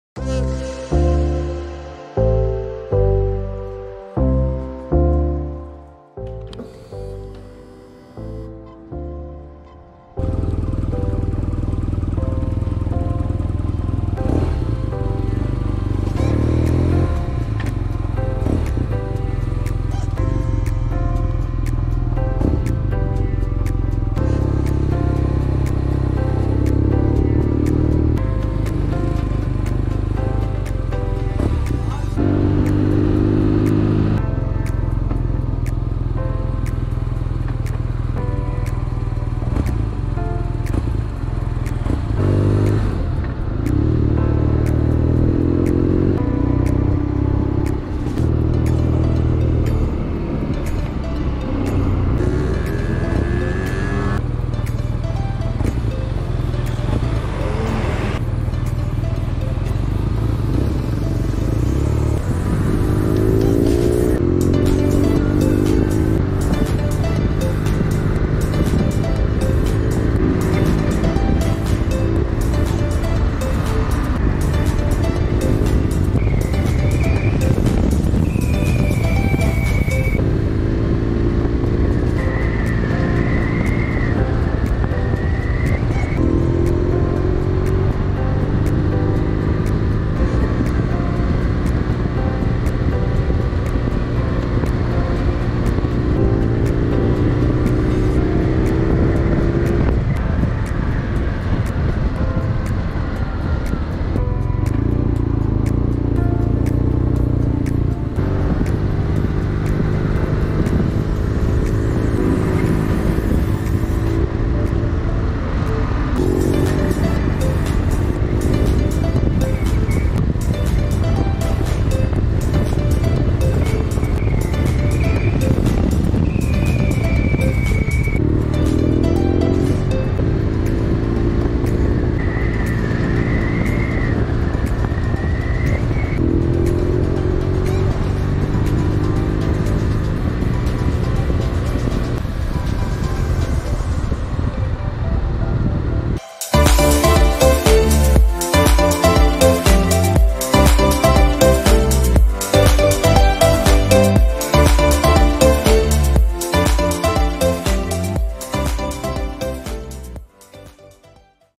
Chasing The Sunset BMW R NineT Scrambler Option 719.
Riding with BMW R nine T Scrambler Options 719. (HyperSmooth GoPro Hero 7 Black).